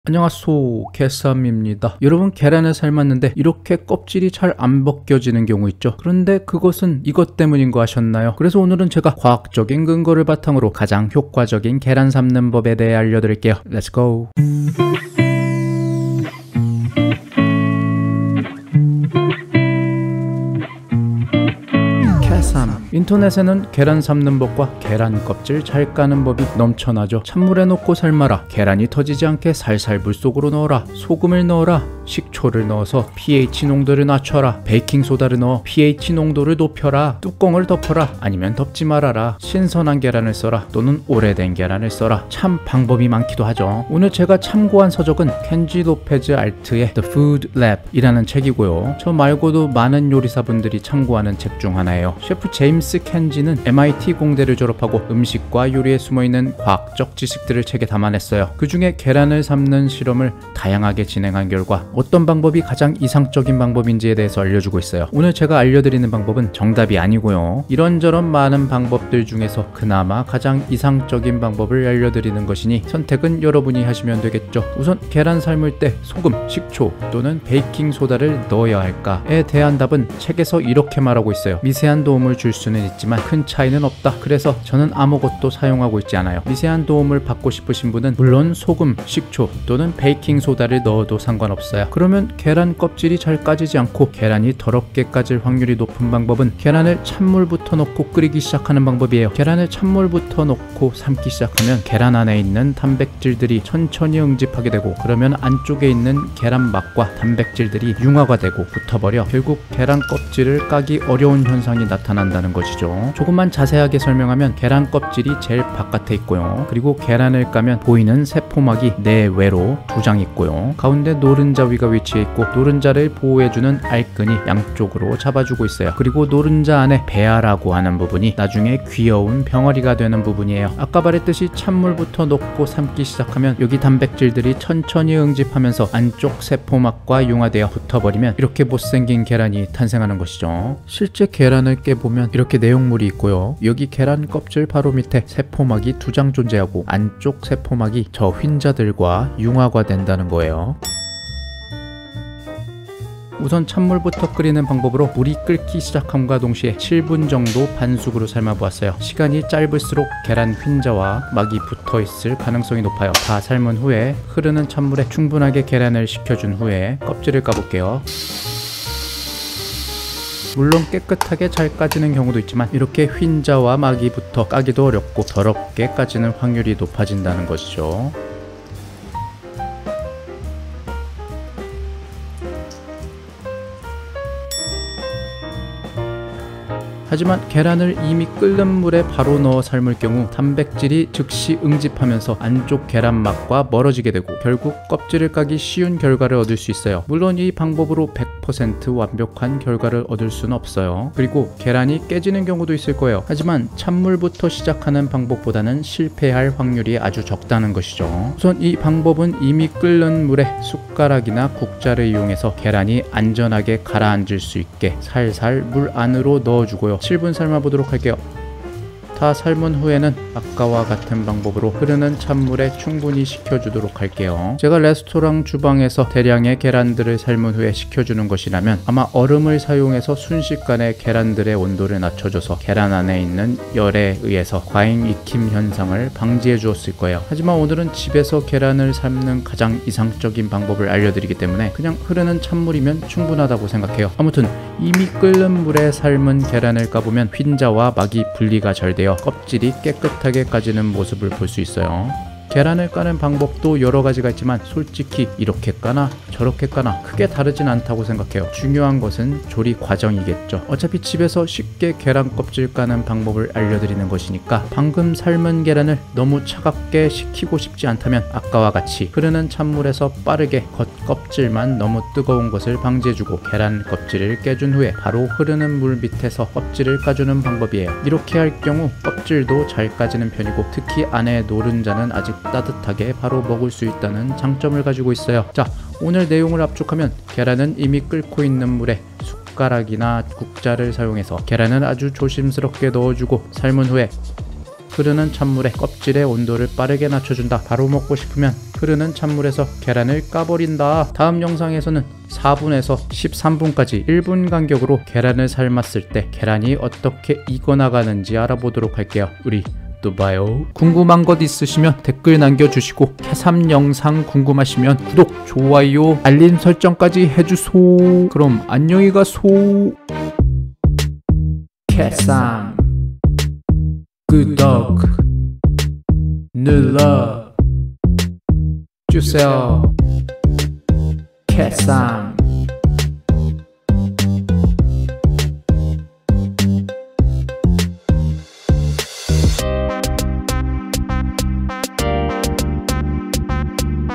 계란 삶는법 :: 계란 못 삶는법 :: 계란 삶을때 껍질 잘 까지게 하려면 ::
안녕하세요 , 캐삼 입니다.
오늘은 계란 껍질이 잘 까지게 계란 삶는 법 알려드릴게요!
당신의 소중한 시간, 제 영상 시청 너무 감사합니다.

영상에서 사용된 상품 정보

링크된 제품들은 영상에 나온 것과 같은 브랜드의 상품은 맞으나, 구입처는 쿠팡이 아님을 알려드립니다.
관심 있으신 분들은 충분히 고려하신 후 구입하시길 바랍니다.
감사합니다!

-자료출처
THE FOOD LAB – J. KENJI LOPEZ ALT
음식과 요리 - 해럴드 맥기(Harold McGee)